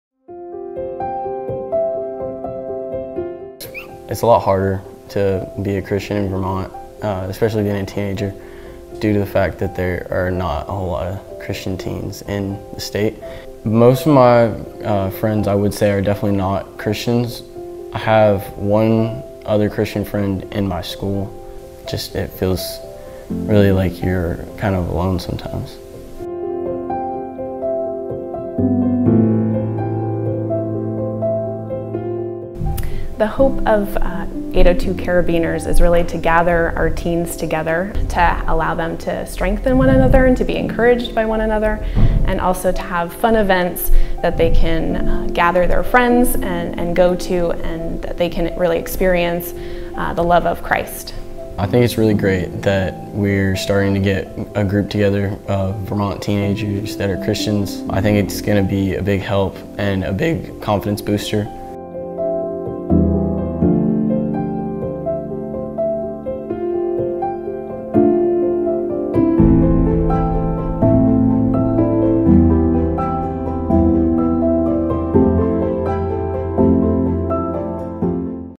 NEME 2019: Vermont Youth Gatherings (French Subtitles)
A group of parents and volunteers are working together to help youth from around one of America's least religious states connect to Christ and to each other.